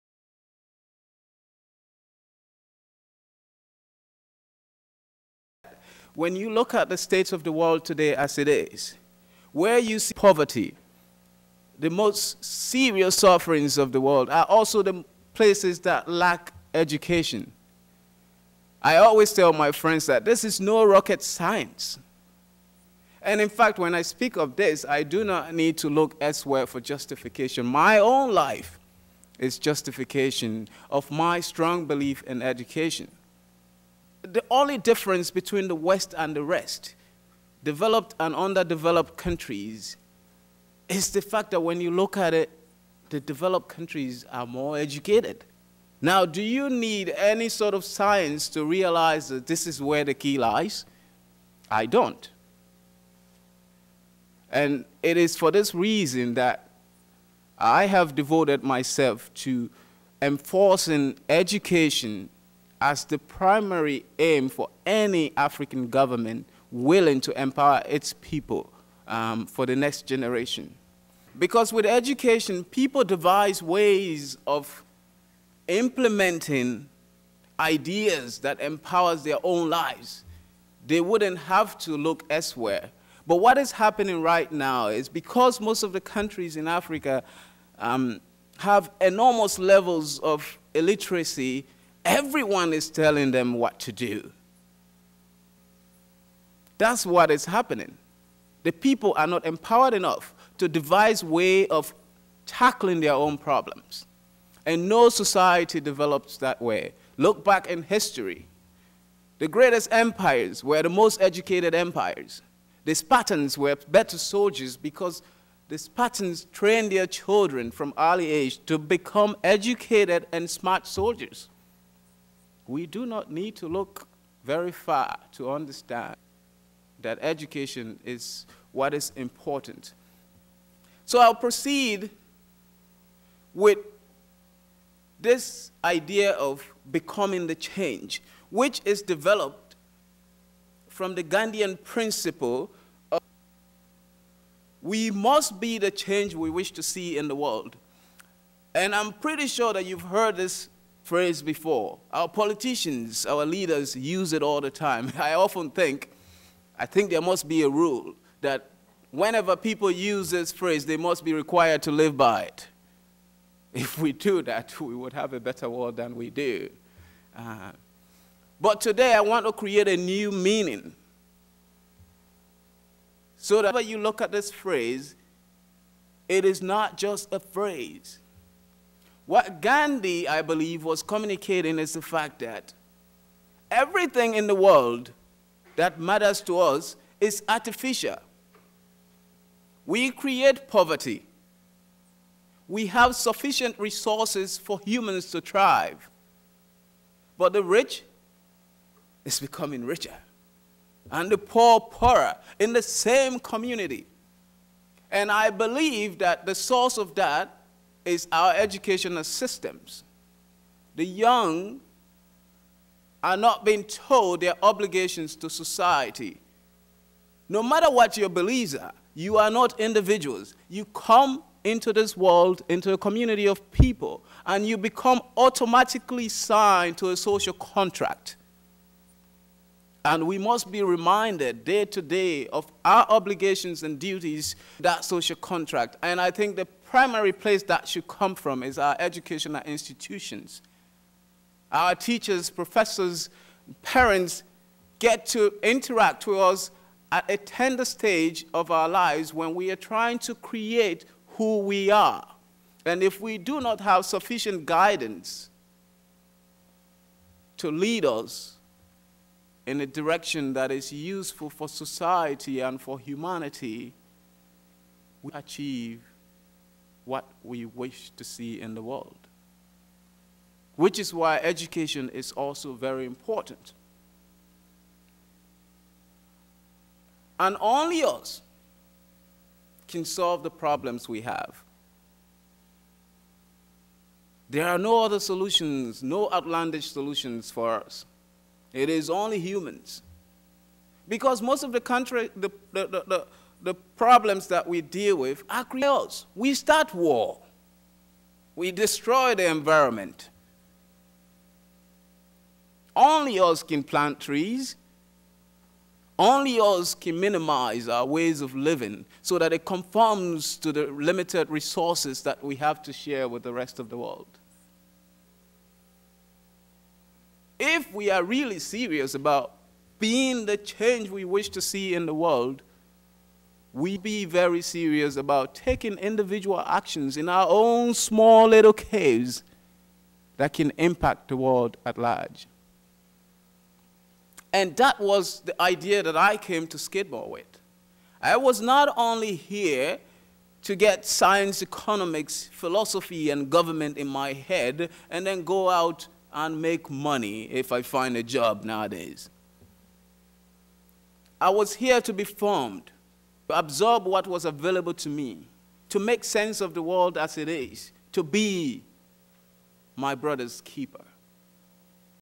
JenPro Skidmore Presentation
Joseph Kaifala presents about becoming the change and the idea of creative thought matters at Skidmore College.